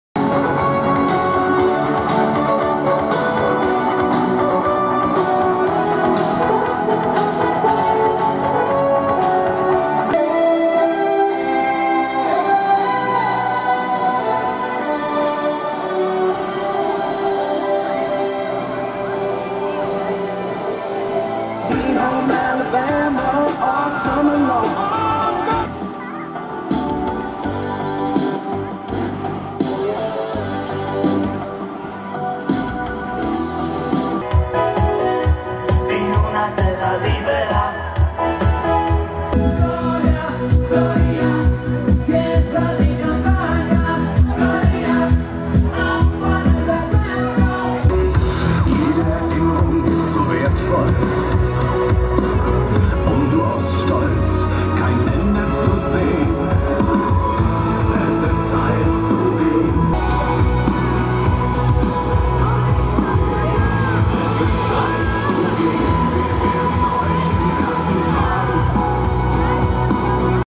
NightGlow
Night glow der Ballone in Achenkirch am Achensee
am 6. März 2018